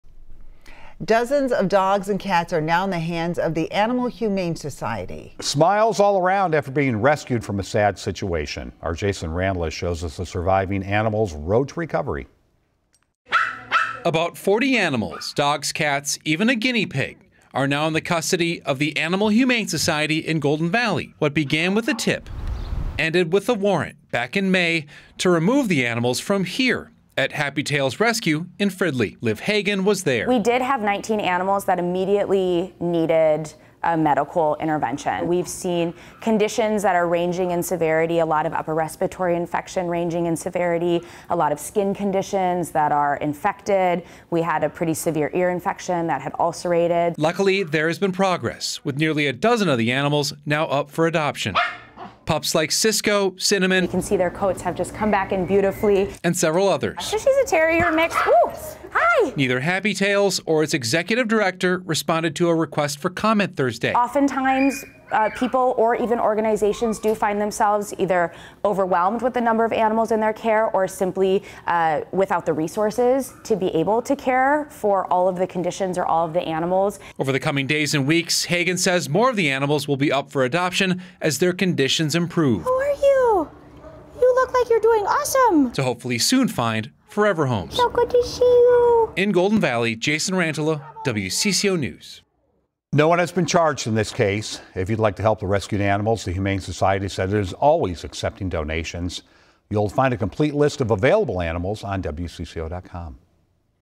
Animal Humane Society looks to rehome animals taken from shelter under investigation
Earlier this month, nearly 40 animals were rescued from Happy Tails shelter following alleged mistreatment. Now, those animals are thriving at the Animal Humane Society and are looking for their forever homes.